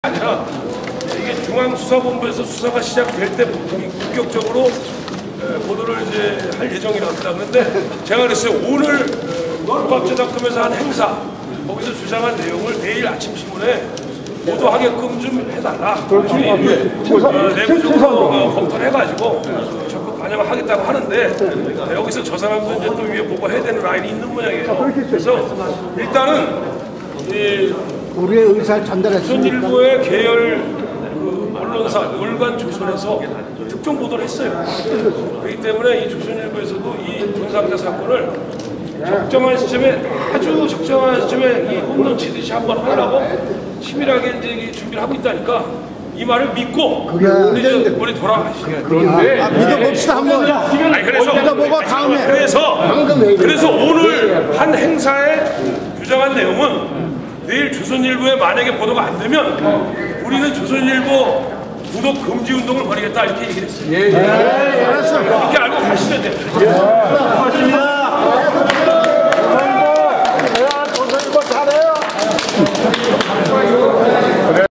[성난 시민들, 조선일보 항의방문] 봉태홍 대표 : 면담 결과 보고
2012년 2월16일 오후 조선일보사 로비. 노무현 가족이 연루된 것으로 추정된 13억 돈상자를 보도 않는데 대한 항의방문.